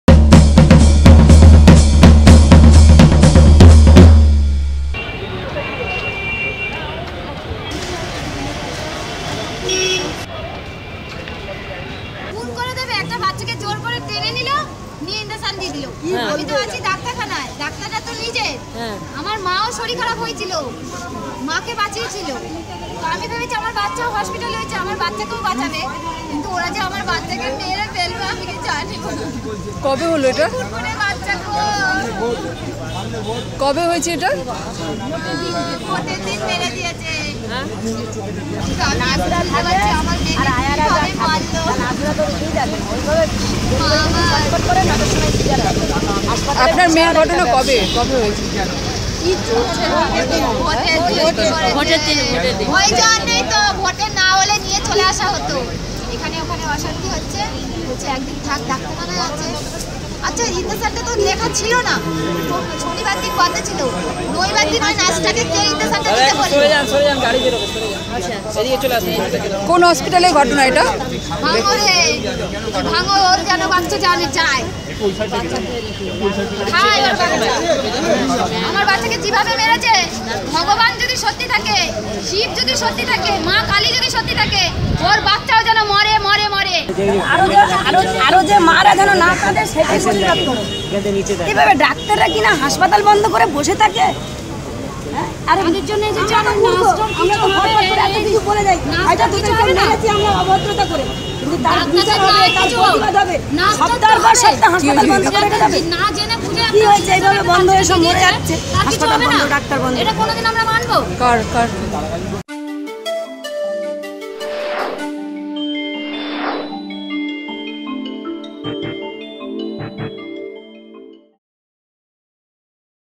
ভাইরাল দিদি আজ কেন NRS এ জুনিয়ার ডাক্তারদের আন্দোলনে তার পুরনো কথা তুলে ধরছেন, শুনুন তিনি কী বলছেন।
ভোটের সময় তার মেয়ে বাঙুর হাসপাতালে মারা যায় সেই কারনে তিনি আজকে  NRS এ এসে অভিযোগ করছেন যে ডাক্তাররা তার মেয়ে কে মেরে ফেলেছেন। তবুও আজ কেন এই বিষয়টি  এখানে তুলে ধরার চেষ্টা করছেন। সেটা একটি ভাববার বিষয় তবুও আমরা তার সাথে কথা বলেছি, তার কথা জানার চেষ্টা করেছি ।